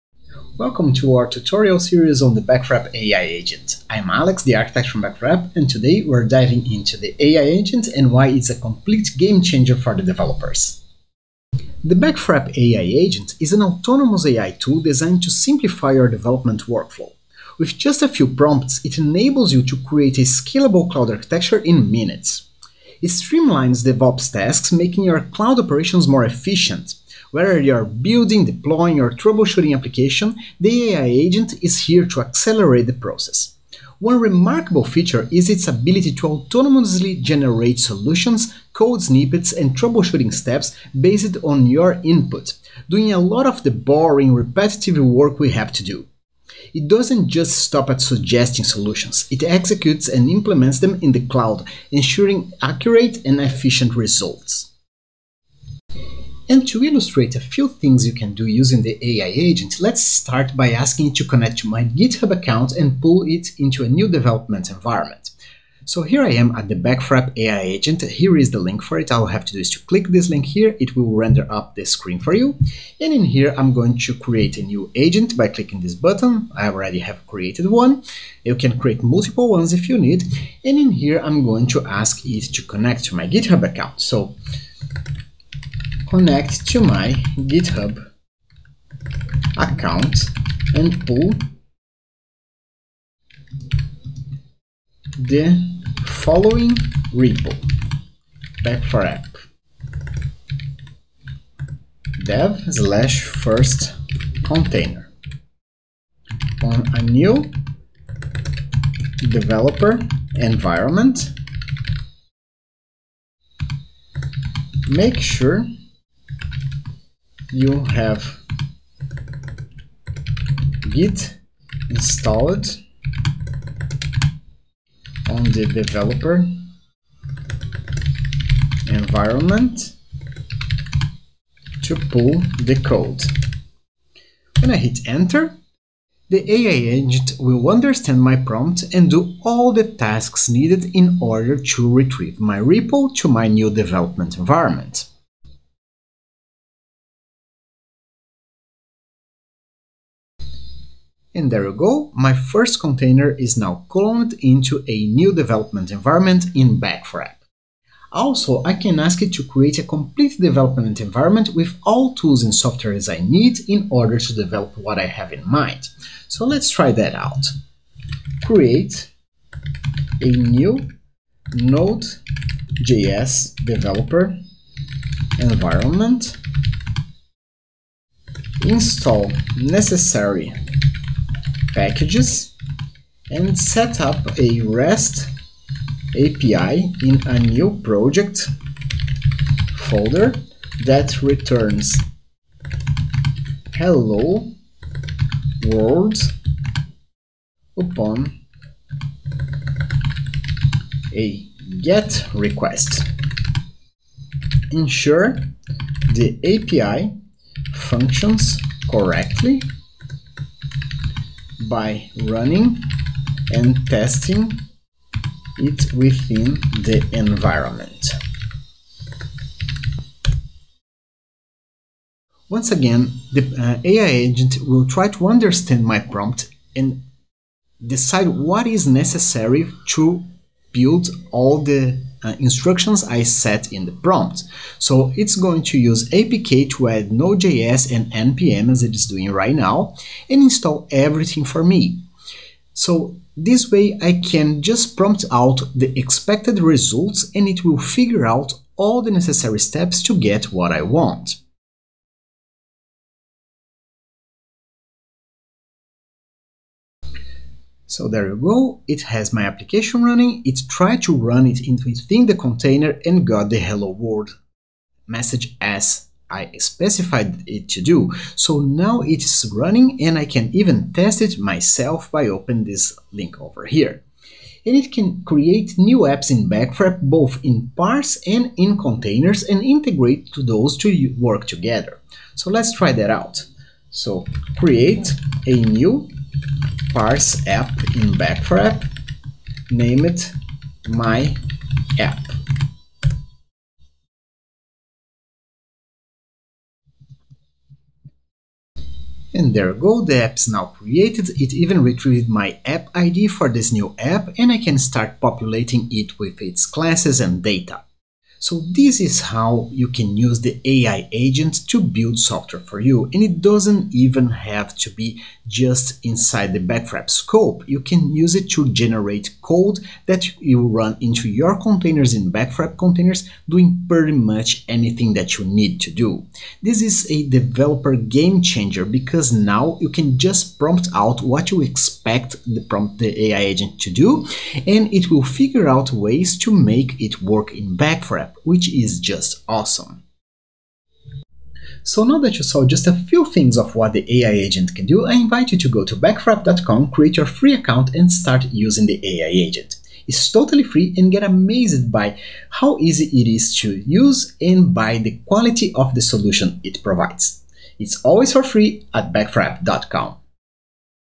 Getting started with Back4app AI Agent
Here's your guide on how to get started using Back4App AI Agent.
Whether you're new to the platform or just looking for a refresher, this guide will help you start using Back4app's AI Agent to simplify your development workflow.

Let's dive in and unlock the full potential of Back4App AI Agent together!